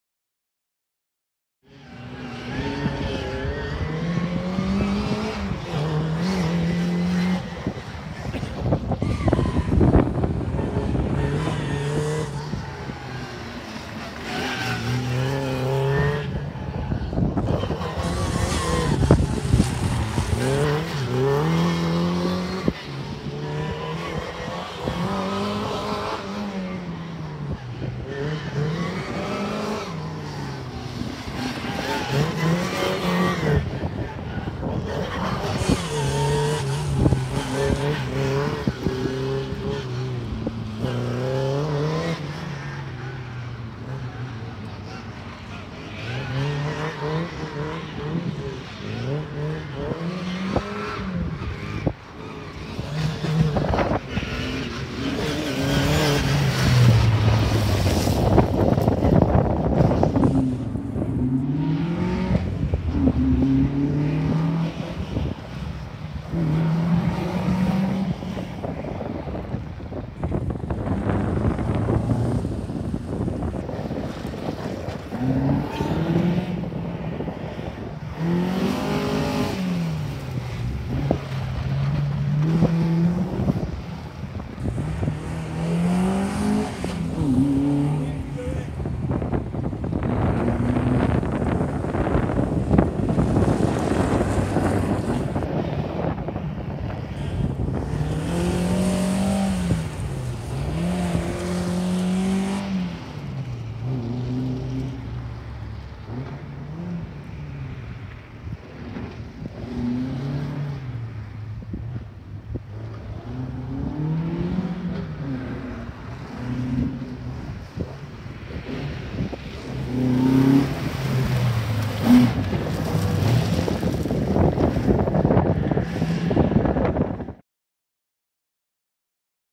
RAC Rally Of The Tests 2018 - Day 3 Test one
It’s all to play for at the top between the leading two on the final day of RACRally18. Who will take the overall honours is anyone’s guess. Here they are tackling the third test of the morning at Aust Services.